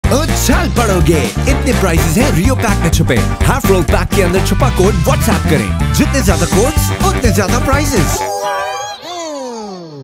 Rio Aur Jeeto- 10sec
Aab har koi jeet sakta hai RIO aur Jeeto kay sath! 🎊
Half roll kay andar chhupay code ko WhatsApp karo aur exciting prizes jeetnay ka mauka hasil karo!